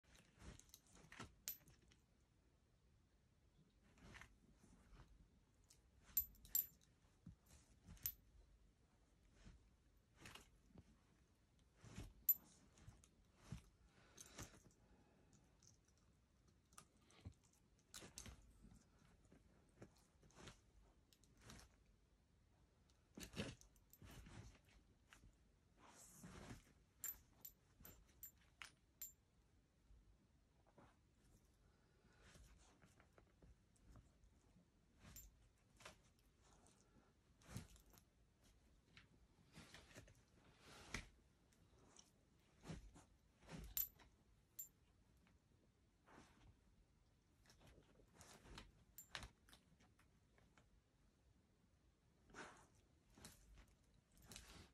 Puppy 🐶 Paddy cleverly burrows his bones on the bed!
Does your Jack Russell do this?